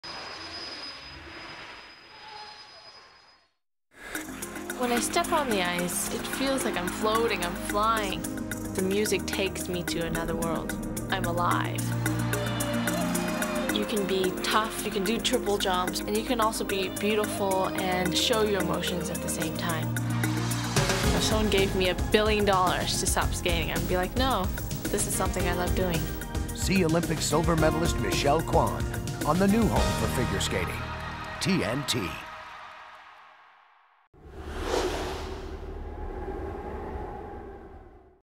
TNT Michelle Kwan promotes Japan Open 1999
Storyville Productions director Glen Owen chose Guillotine Post's founder Michael Koepenick to cut this promo starring Michelle Kwan for the Japan Open on TNT. "This was one of my favorites to cut." says Koepenick. "There was so much great footage to work with. It was shot on film and they let us get really artsy with this one".  This promo was later chosen by Avid Technologies to represent the Avid Symphony at the NAB convention in 1999 and won several industry awards as well.